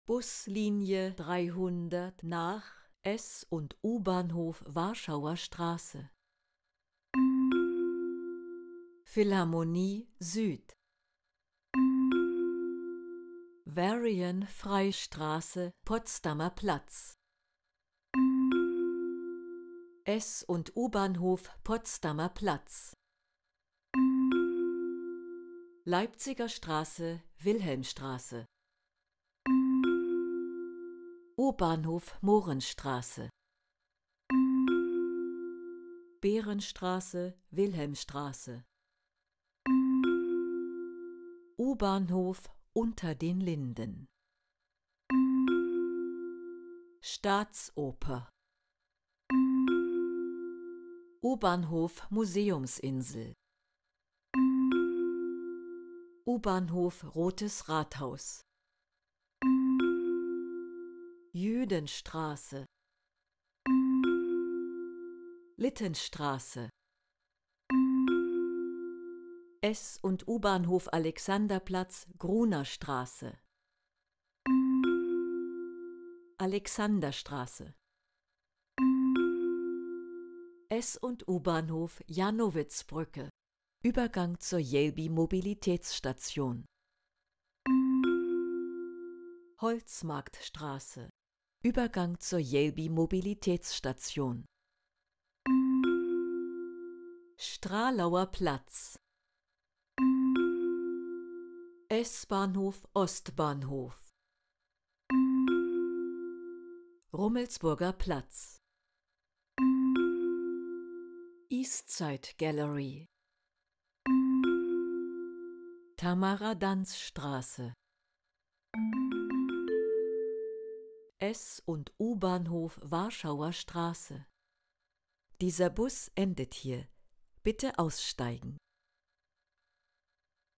Linienverlauf: 300 nach S+U Warschauer Str. (Berlin) [4K]
Der Linienverlauf mit Ansagen der Bus-Linie 191 auf dem Weg von Philharmonie Süd nach S+U Warschauer Str.

𝐇𝐚𝐥𝐭𝐞𝐬𝐭𝐞𝐥𝐥𝐞𝐧:
Philharmonie Süd
Varian-Fry-Str./Potsdamer Platz
S+U Potsdamer Platz
Leipziger Str./Wilhelmstr.
U Mohrenstr.
Behrenstr./Wilhelmstr.
U Unter den Linden
Staatsoper
U Museumsinsel
U Rotes Rathaus
Jüdenstr.
Littenstr.
S+U Alexanderplatz/Grunerstr.
Alexanderstr.
S+U Jannowitzbrücke
Holzmarktstr.
Stralauer Platz
S Ostbahnhof
Rummelsburger Platz
East Side Gallery
Tamara-Danz-Str.
S+U Warschauer Str.

𝐖𝐢𝐜𝐡𝐭𝐢𝐠: Das Bild- und Tonmaterial ist lediglich an die Fahrgastinformation der BVG angelehnt.